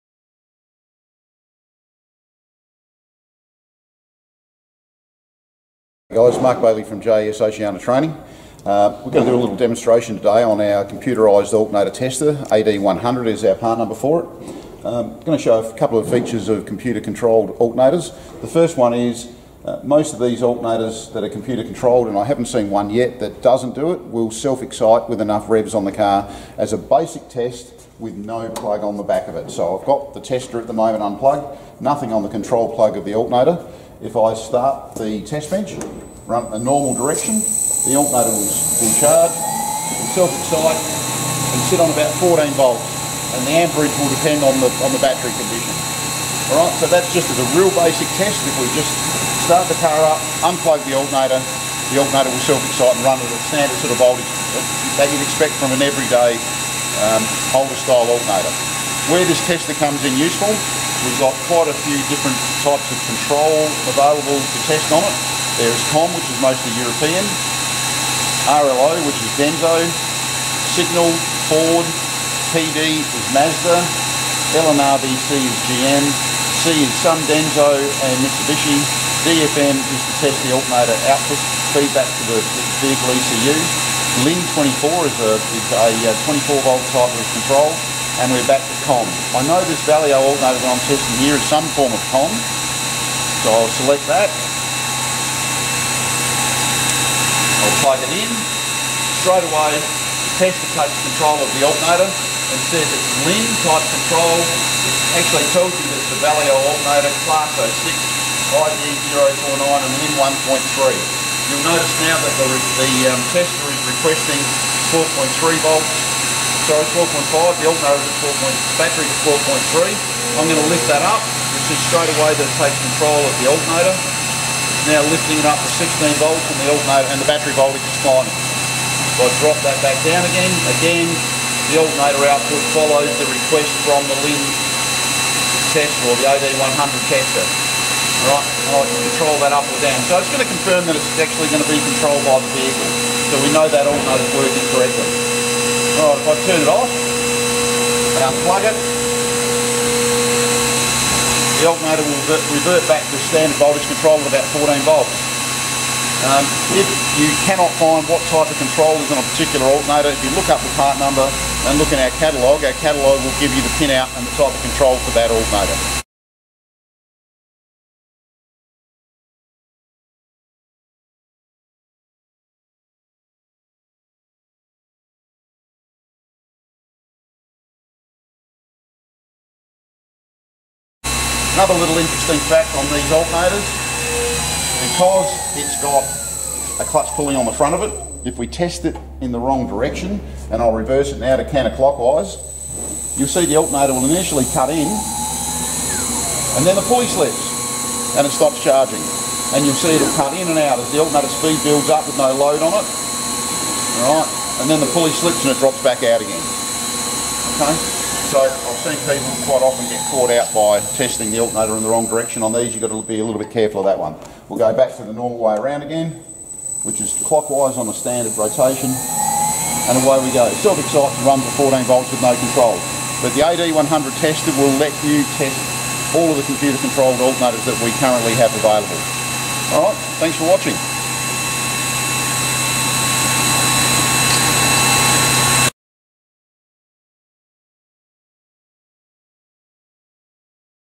JAS Oceania AD100+ Computerised Alternator Tester
The AD100+ JAS Alternator tester allows the Automotive Technician to test an ECU controlled alternator without removing it from a vehicle. This will save workshop time and incorrect diagnosis.